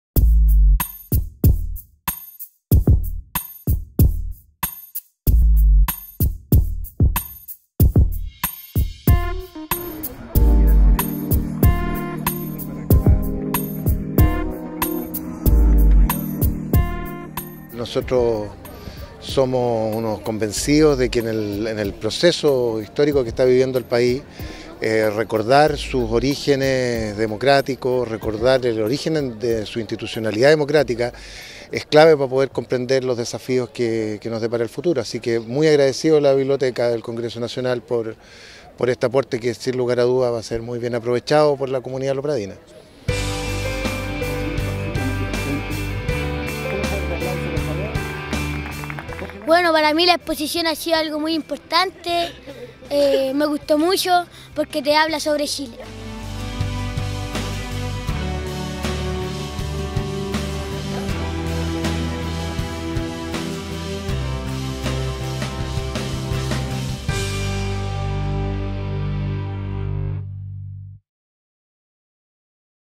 BCN inaugura exposición itinerante "Historia del Congreso Nacional" en Lo Prado
Coincidentemente con la conmemoración de los 205 años del Congreso Nacional, este lunes 4 de julio de 2016 la BCN inauguró en la Municipalidad de Lo Prado la exposición itinerante "Historia del Congreso Nacional", que presenta a la ciudadanía los hitos más destacados del parlamento chileno por medio de interesantes datos y hechos anecdóticos.